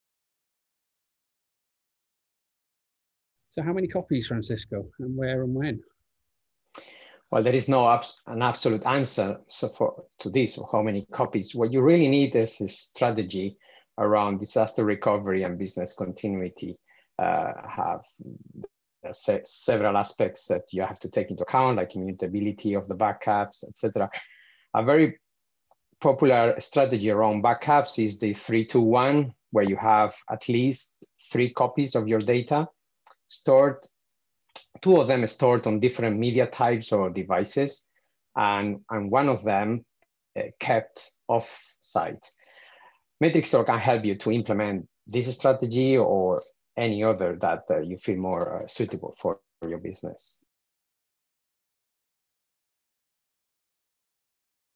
Security Webinar 2021 (Clip 5)
Global Sales Engineer, Mark Habberfield, along with Object Matrix CTO, Francisco Ontoso-Ramos, discuss and demonstrate some of the best ways to protect your valuable media assets.